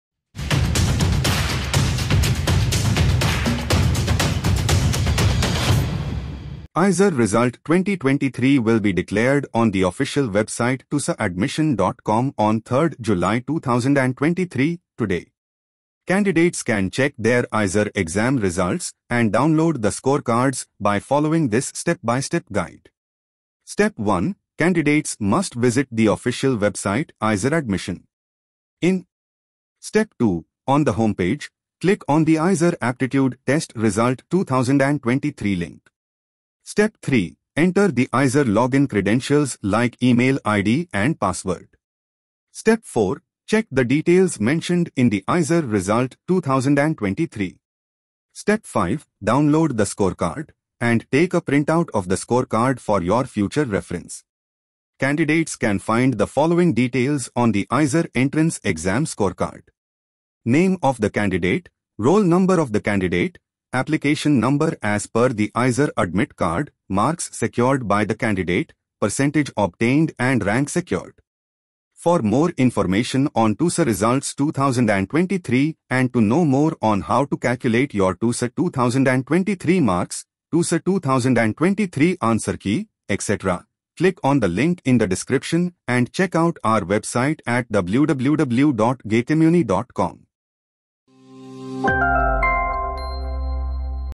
How To Check & Download IISER Aptitude Test Results 2023?| GetMyUni
IISER Result 2023 will be declared on the official website iiseradmission.com. IISER Aptitude Test 2023 result date has been announced, and the results will release on Jul 3, 2023. Students can view the IISER IAT 2023 Result by entering their roll number and password.

Check out the video to know more about How to IISER 2023 Results, Details Mentioned On IISER 2023 Results, IISER 2023 Counselling, IISER 2023 Toppers, IISER 2023 Rank List etc

Time Codes
00:00 - Introduction
00:06  - GMU Intro
00:10 - How To Download IISER 2023 Results?
01:20 - Conclusion

Everything you need to know about Courses